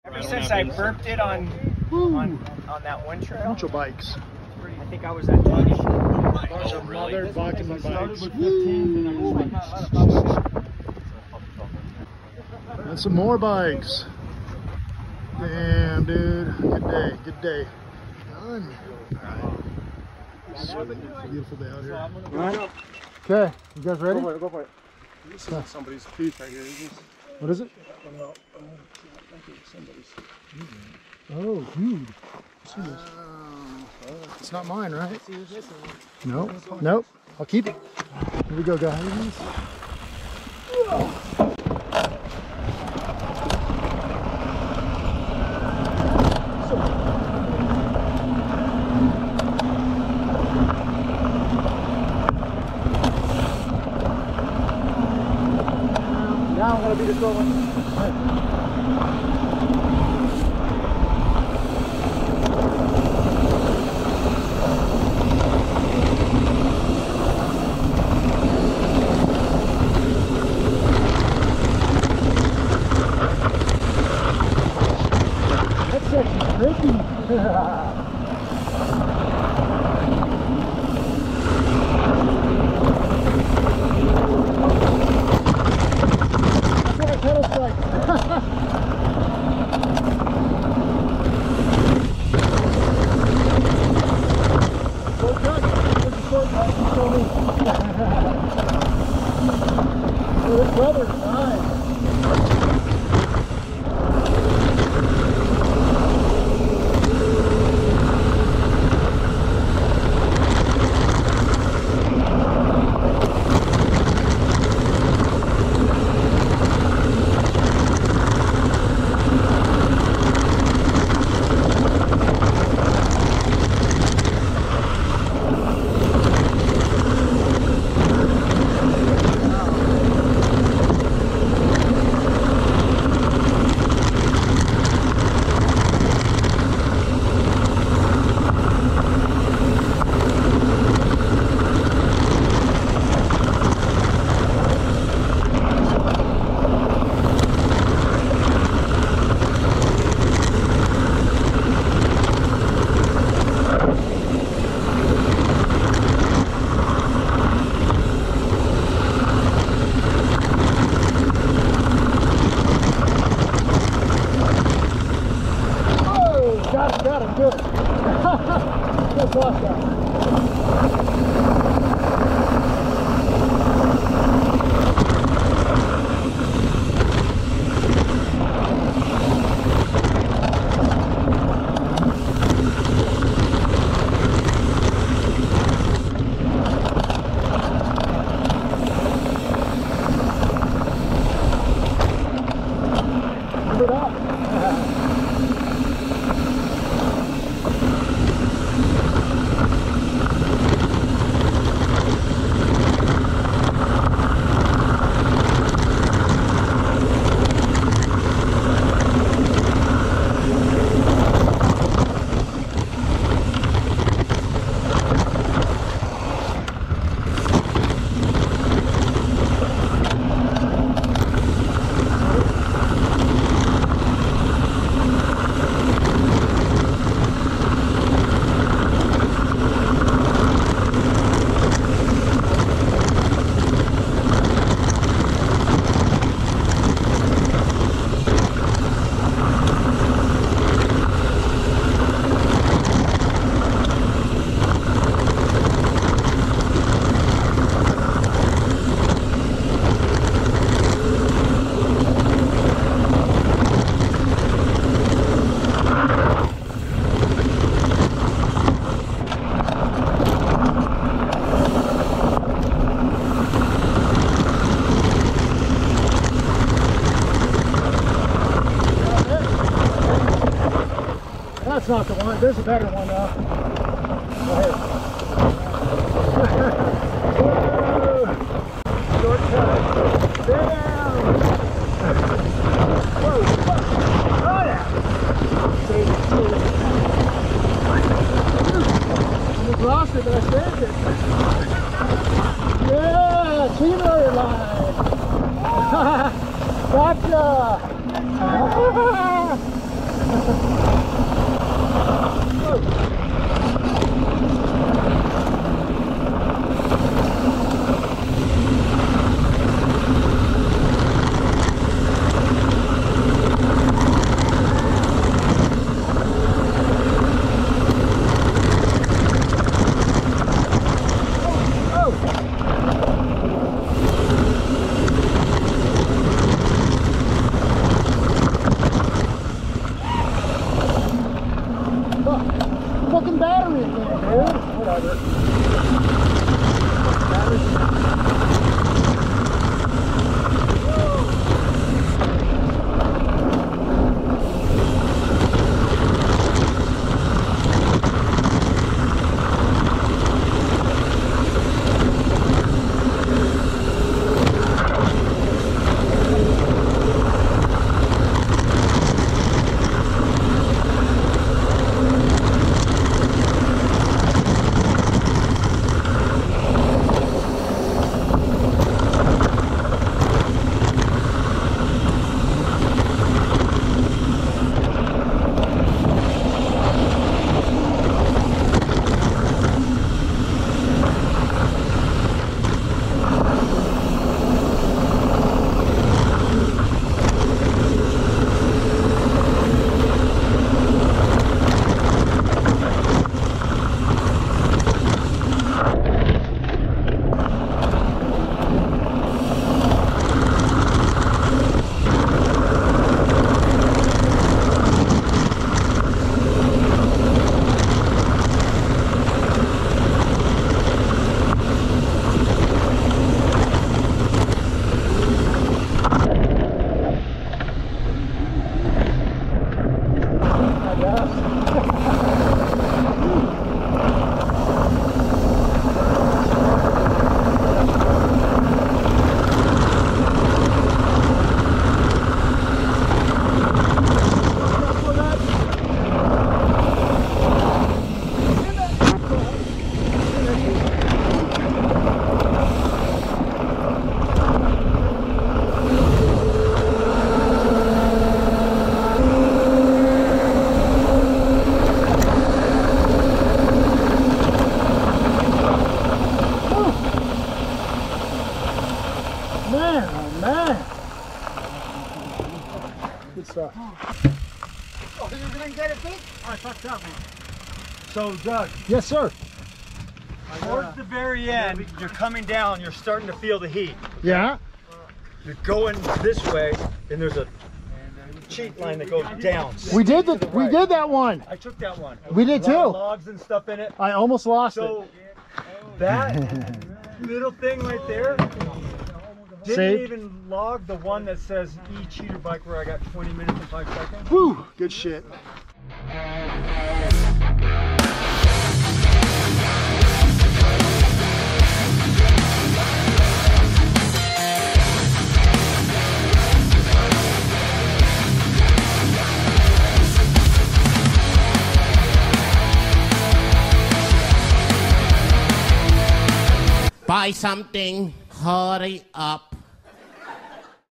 7 Mile Downhill, Mt Pinos, CA
Mt Pinos in Frazier Park, CA. 7 miles of downhill trail. Fast and Flowy.